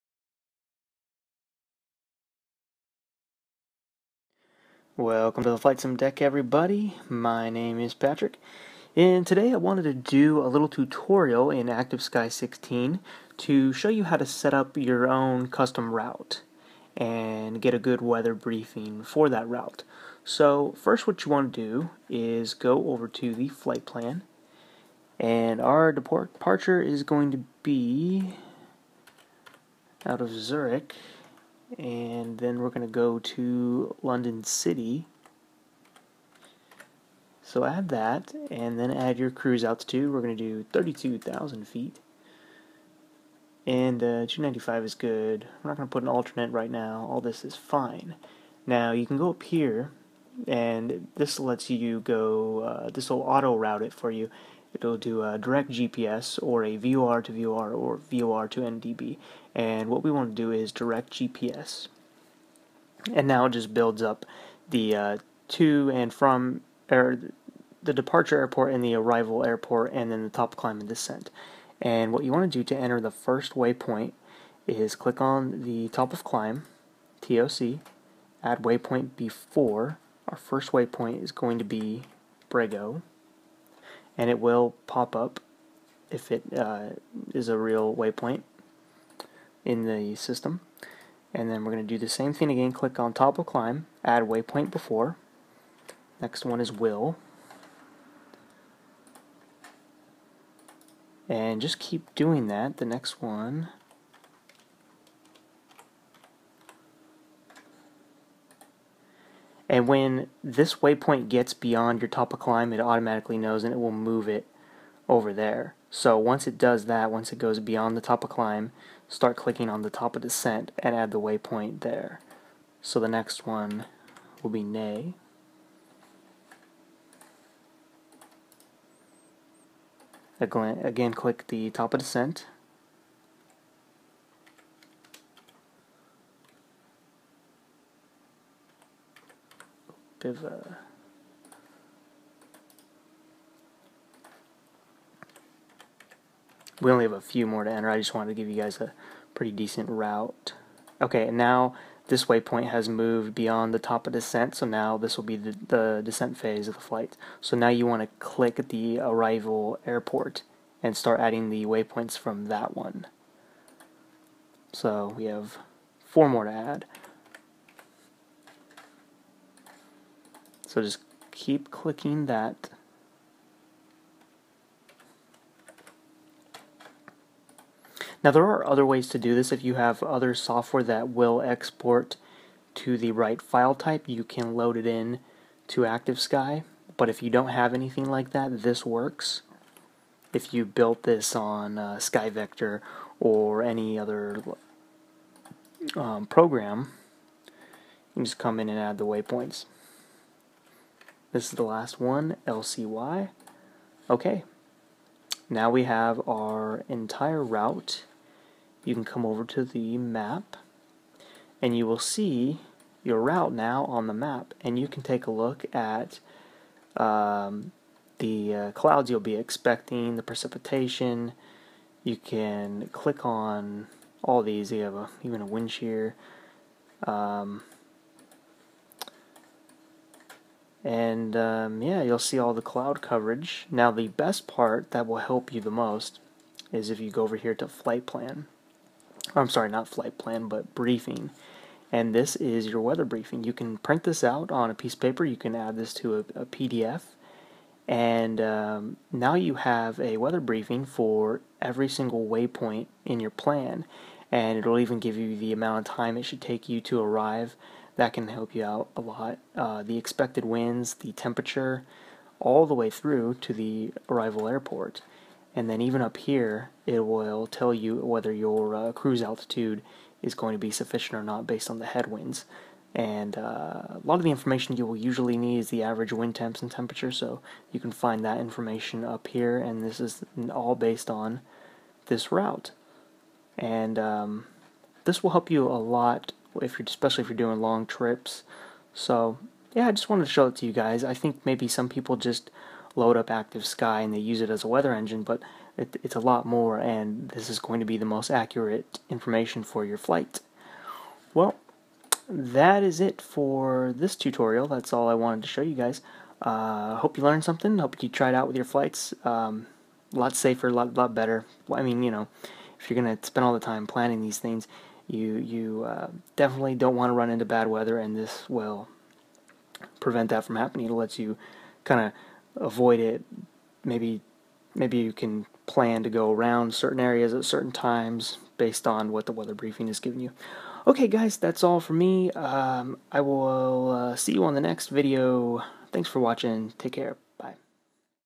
How To | Active Sky 2016 Planning a Custom Route
In this tutorial I show you how to add your own way points in Active Sky 2016. From there we take a look at the weather briefing.

Software:
Adobe Premiere Pro CC15
Nvidia Shadow Play
SkyVector.com
Hardware:
i7 4790k Quadcore oc 4.3ghz
Geforce GTX Titan 12GB
16gb RAM 2400mhz
2TB Hard Drive
Windows 10
Saitek Rudder pedals, throttle, and yoke
Add ons:
Orbx Global
Orbx Vector
Orbx openLC North America
Orbx Trees HD
Active Sky 16
Rex 4 Texture Direct w/ Soft Clouds
EzDok v1.18